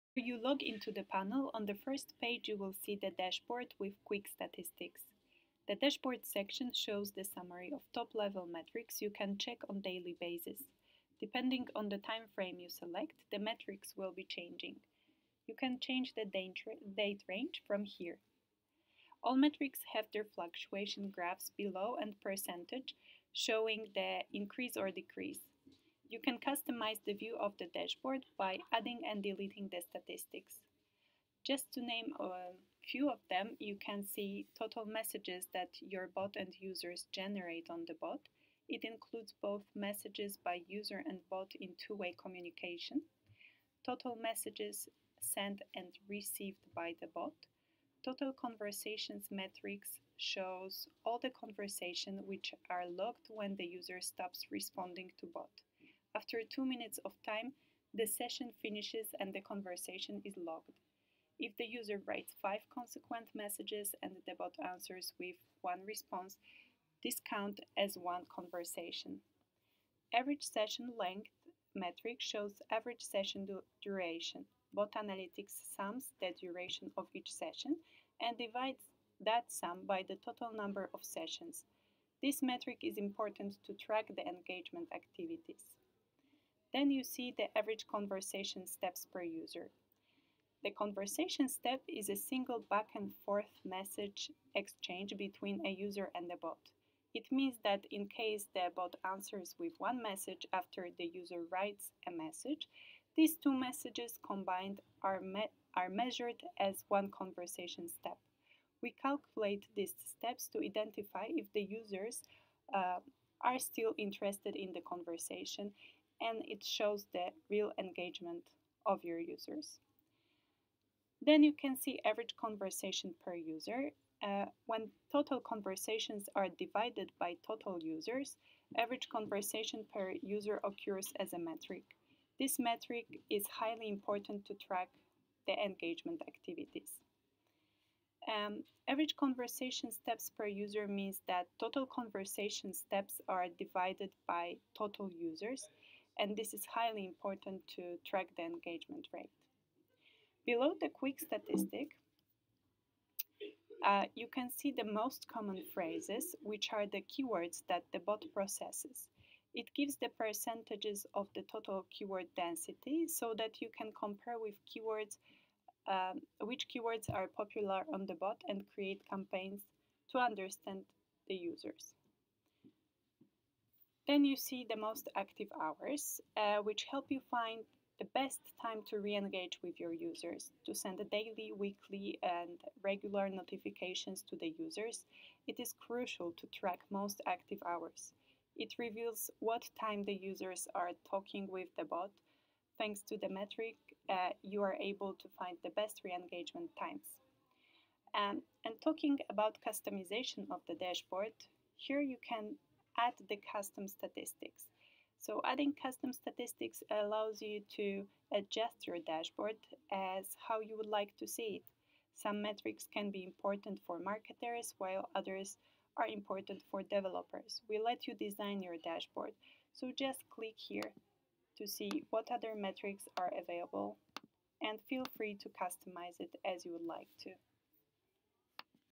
Botanalytics Tutorials: Dashboard
Botanalytics is a conversational analytics tool for bots. Dashboard section shows top-level metrics as your platform generates messages and conversations.

Total messages: All messages that your bot and users generate on the bot. It includes both messages by user & bot in two-way communication. It shows the messages after the integration date.

Total conversations: A conversation is logged when the user stops responding to bot. After two minutes of time, the session finishes and the conversation is logged. If the user writes 5 consequent messages, and the bot answers with one response, this counts as one conversation.

Average conversation/user: When total conversations are divided by total users, average conversation per user occurs as a metric. This metrics is highly important to track engagement activities.

Average conversation steps/user: “Conversation Step” is a single back-and-forth message exchange between a user and a bot. It means that in case the bot answers with one message after the user writes a message, these two messages combined are measured as ‘one conversation step’. We calculate these steps to identify if the user are not interested in the conversation from some point.

Total user: All users that the bot has for all times.

Total sent: All sent messages that bot generates to send to the user. It doesn’t include the messages from the user.

Total received: All received messages that the user types to the bot. It doesn’t include the messages from the the bot.

Average Session Length: Every conversation lasts till the session finishes. To calculate average session duration, Botanalytics sums the duration of each session and divides that sum by the total number of sessions. This metric is important to track the engagement activities.

Most common phrases: These are the keywords that the bot processes. It gives the percentages of the total keyword density so that you can compare which keywords are popular on the bot and create campaigns and understand the users easily.

Most active hours: To send daily, weekly, regular notifications to the users, it’s crucial to track most active hours. It reveals what time the users are talking with the bot. Thanks to this metric you’re able to find the best re-engagement times.

Adding custom stats: Adding custom metrics allows you to adjust your dashboard as how you would like to see. Some metrics can be important for marketers, some metrics can be important for developers. We let you design your dashboard. Just click and add your metric into your dashboard.